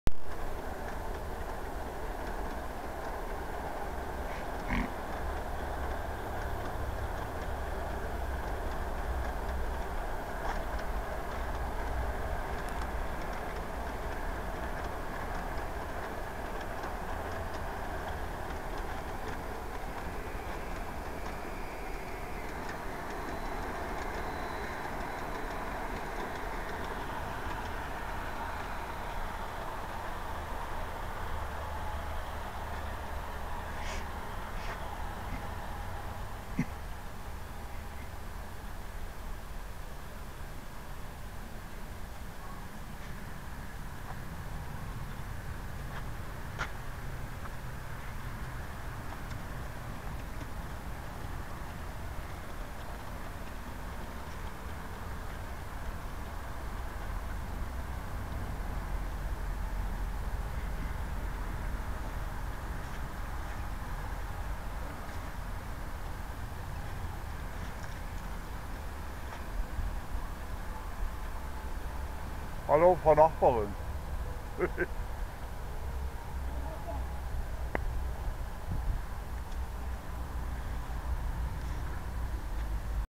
Heikos Testfahrt rueckwaerts mit schwerer Last im Schub
Lok schiebt die Wagen ohne Entgleisungen zuverlaessig ins Depot .